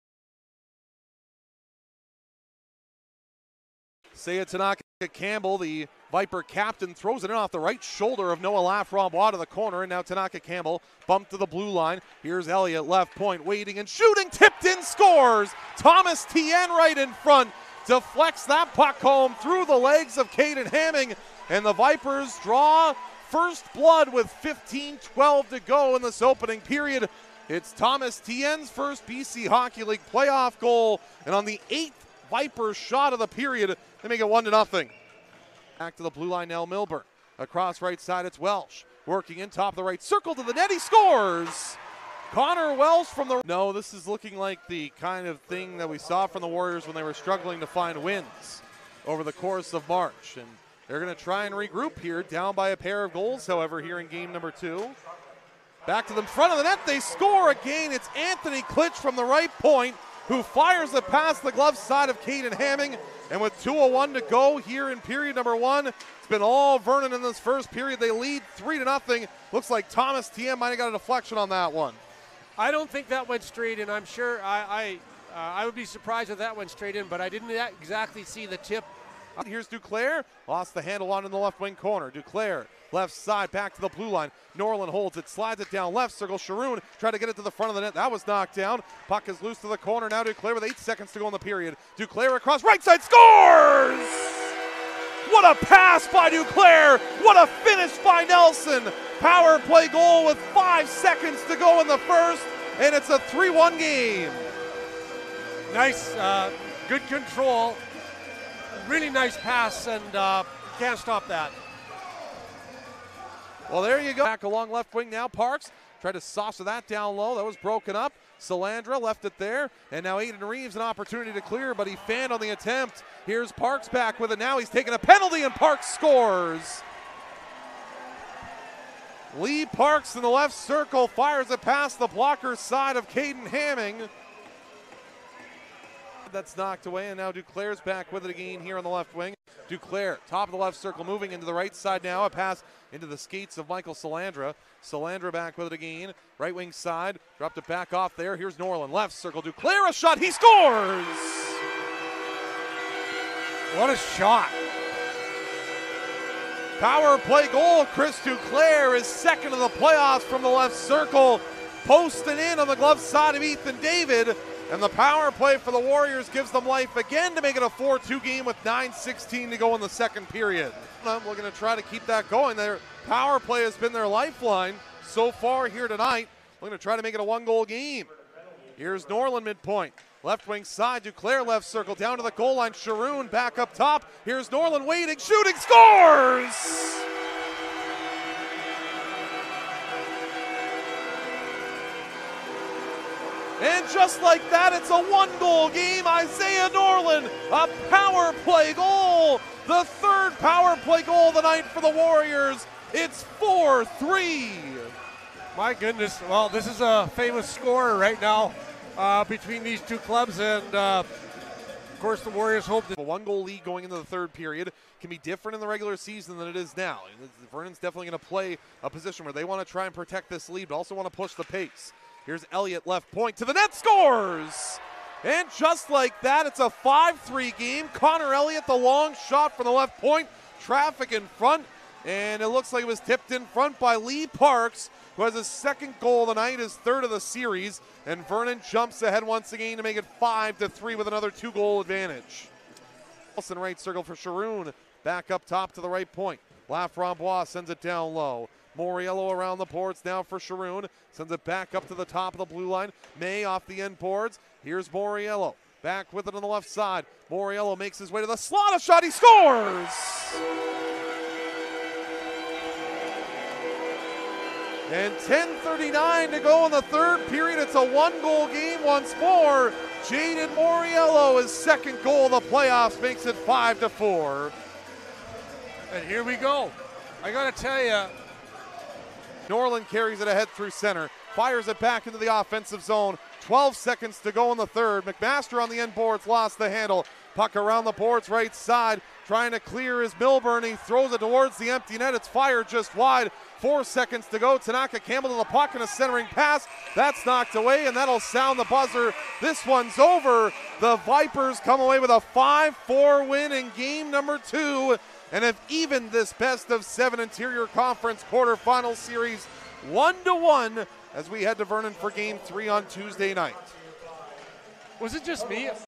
Highlights vs. Vernon (Apr. 1)
The Warriors could never find an equalizer in a back-and-forth Game #2 of their Interior Conference Quarter Final series against the Vernon Vipers, falling by a 5-4 score and seeing the series tied at 1-1.

Post Game Highlights are powered by Nor-Val Rentals!